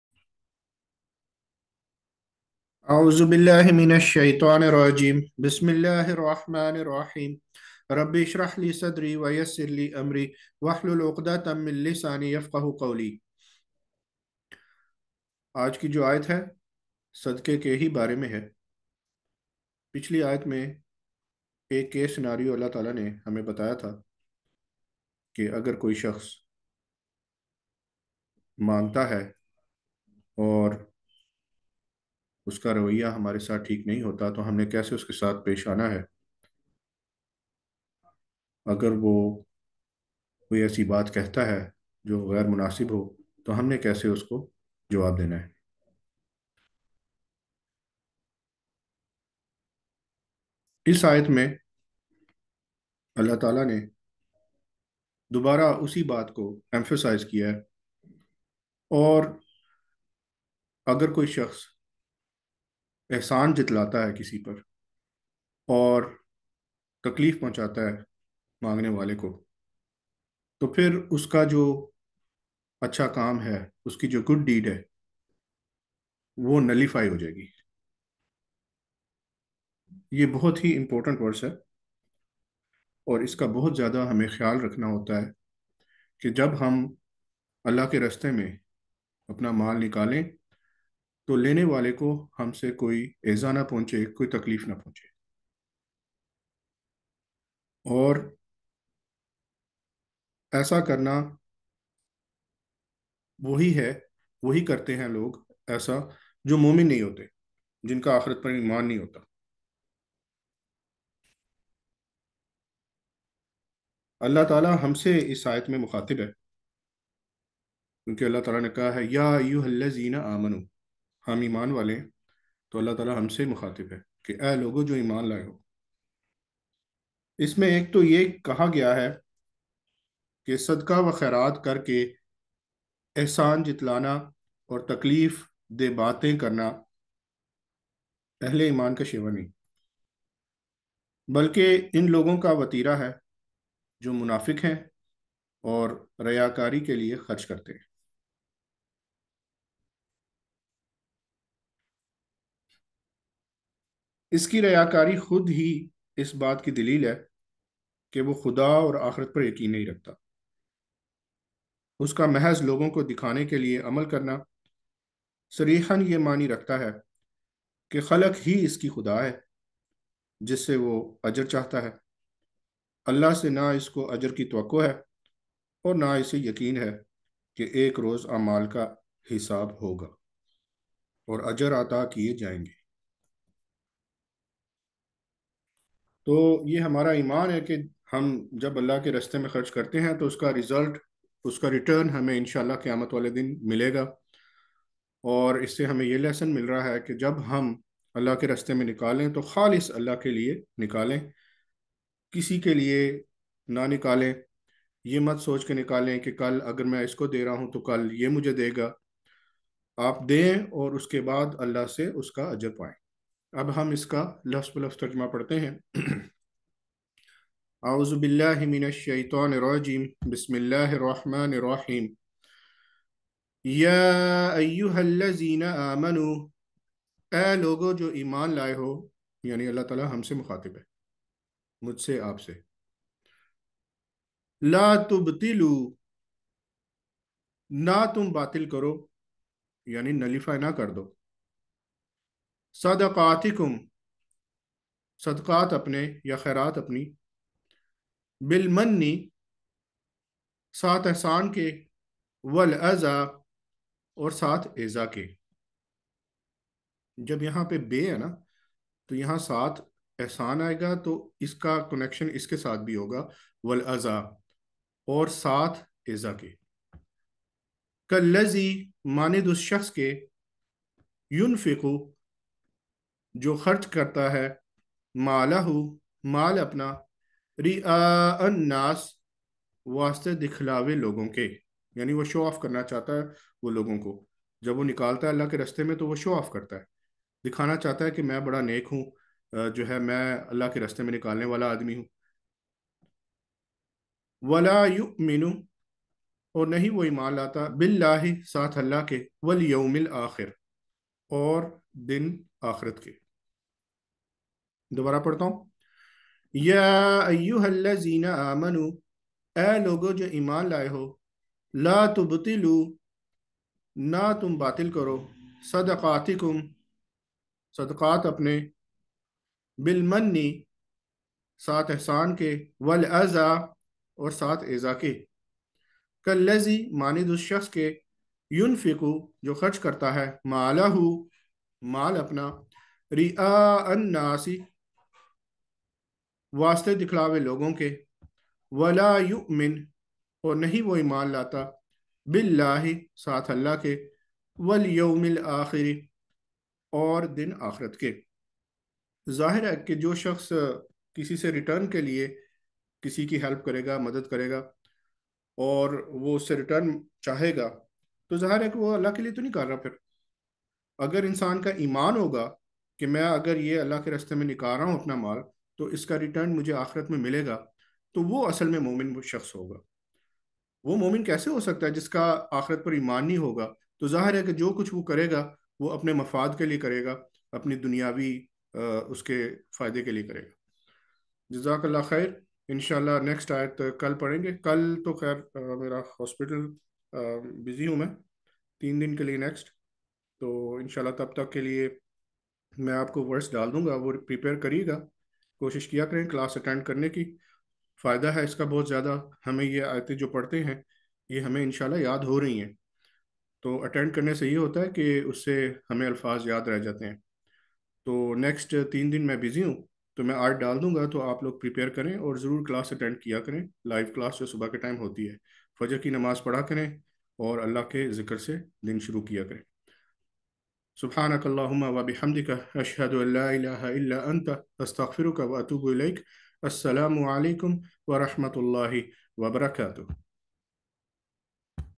Surah Baqarah-2 Verse-264 Part-A Showing off during Sadaqah has no reward in Akhirah
Video from Abdul Waheed Rana